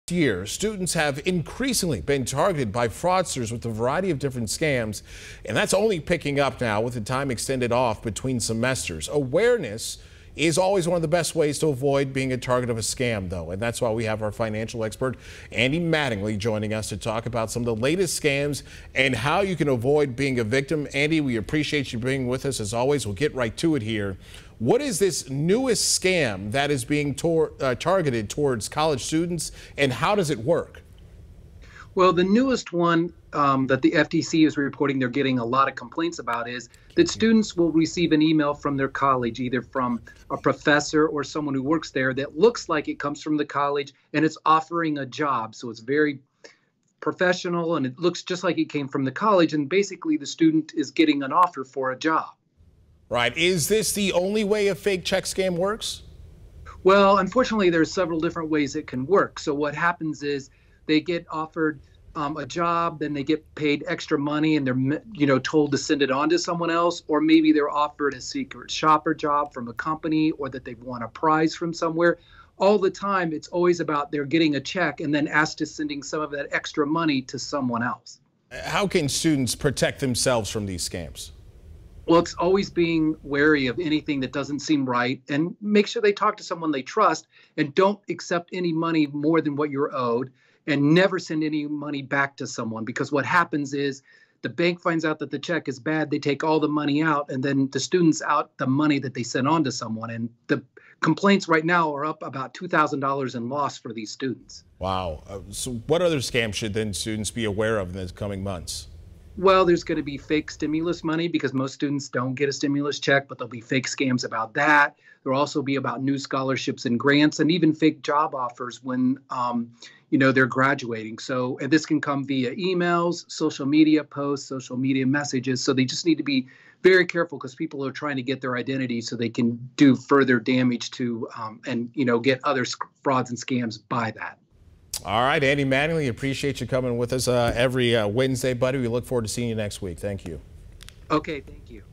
Students beware, fraudsters are targeting you
Over the past year students have increasingly been targeted by fraudsters with a variety of different scams and that is only picking up with the extended time off between semesters. Awareness is always one of the best ways to avoid being a target of a scam.